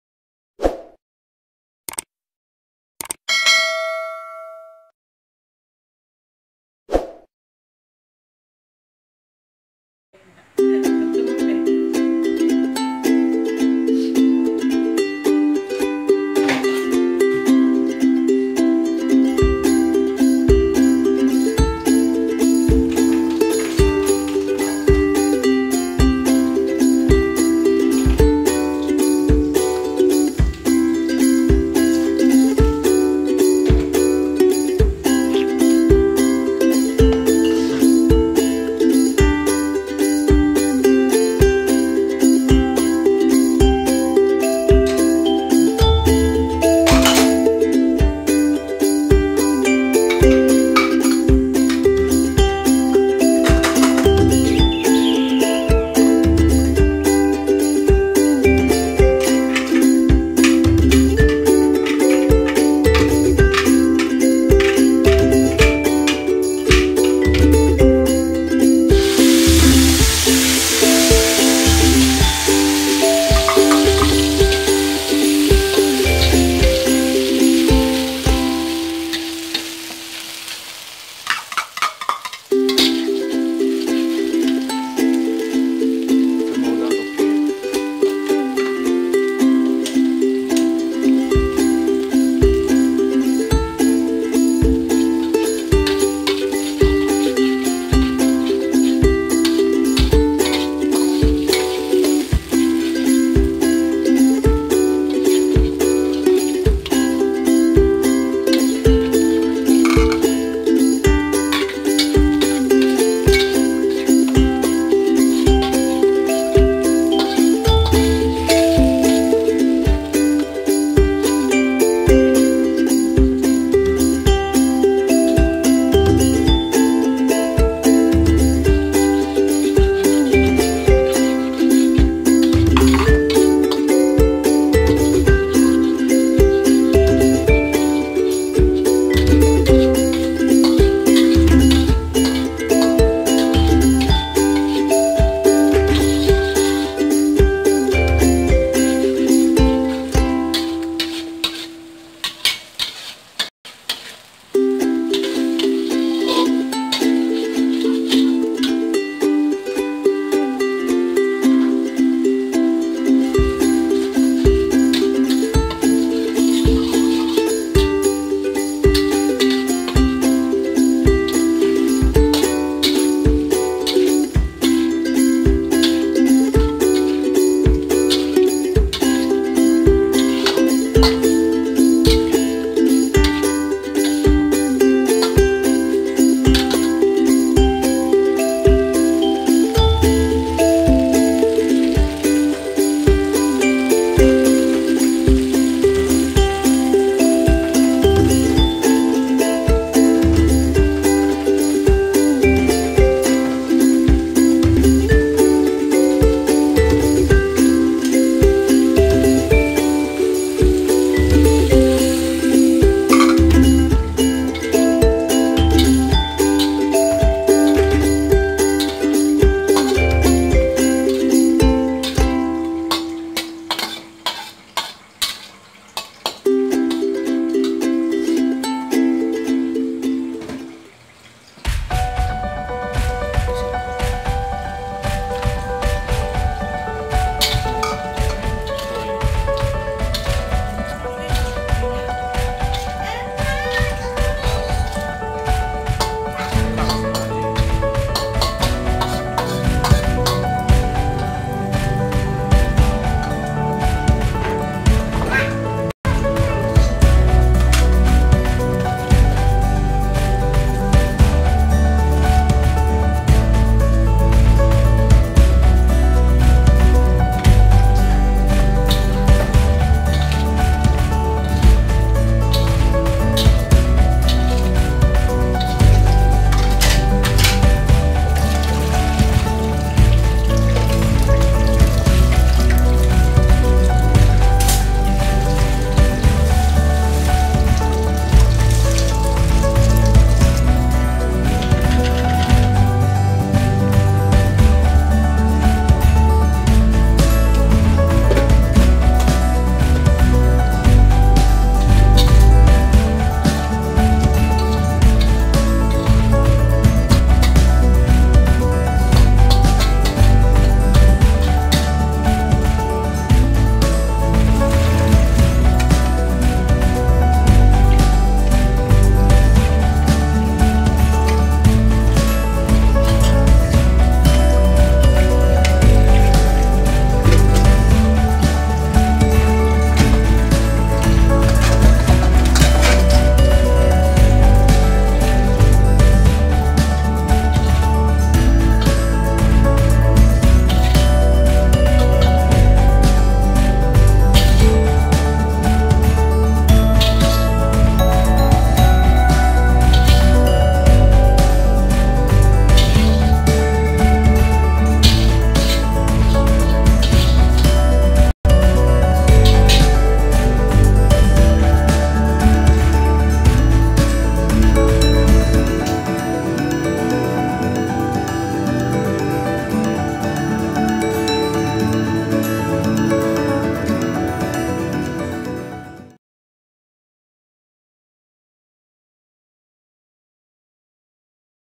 Sambal petai/masakan sendiri sangt senang
Video ini memaparkan cara memasak sambal petai campur udang.Jom tengok dan selamat mencuba.